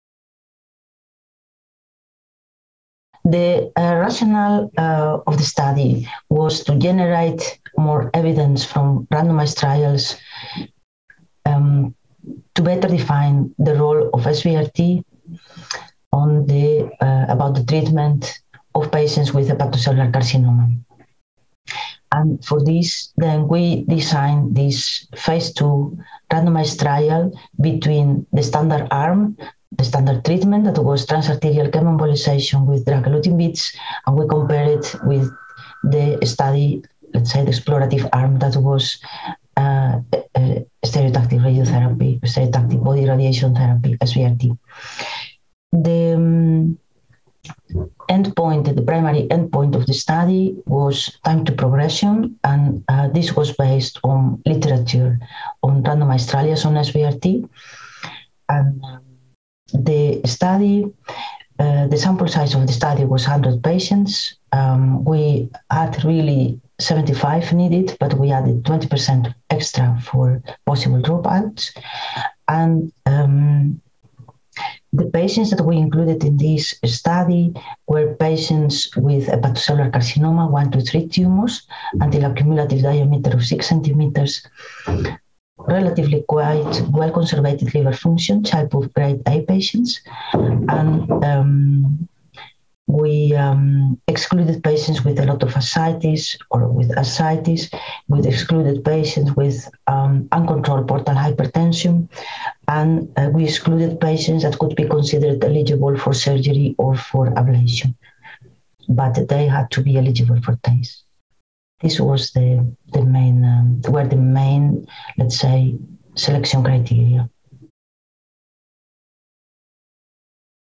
Rationale and study design of Phase II trial comparing TACE and SBRT for hepatocellular carcinoma
Alejandra Mendez Romero, MD, PhD, Erasmus MC Cancer Institute, Rotterdam, Netherlands, discusses the rationale and study design of a randomized Phase II multicenter study (NCT02470533) which compared the use of transarterial chemoembolization with drug-eluting beads to the use of stereotactic body radiation therapy (SBRT) for hepatocellular carcinoma (HCC). The target enrollment of the study was 100 patients, with the primary endpoint being time to progression.

This interview took place at the American Society for Radiation Oncology (ASTRO) 2022 Congress in San Antonio, Tx.